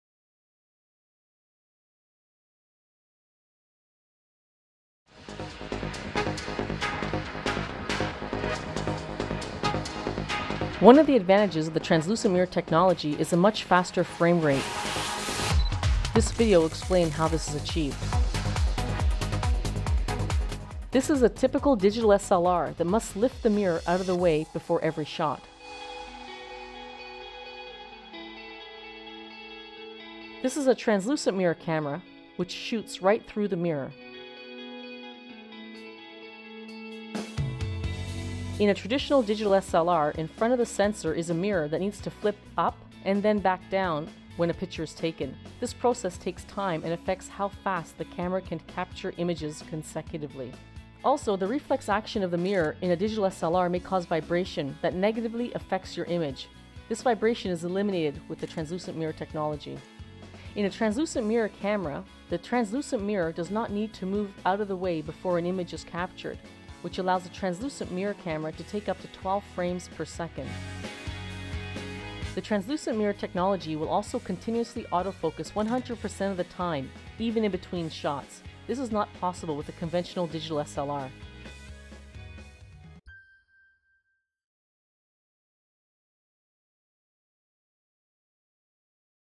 Sony Translucent Mirror Technology vs. SLR
This video compares the unique Translucent Mirror Technology availbale on select Alpha models with conventional SLR. It shows the advantages of Translucent Mirror Technology with regards to frame rate and fast auto focus in both movie and photo mode.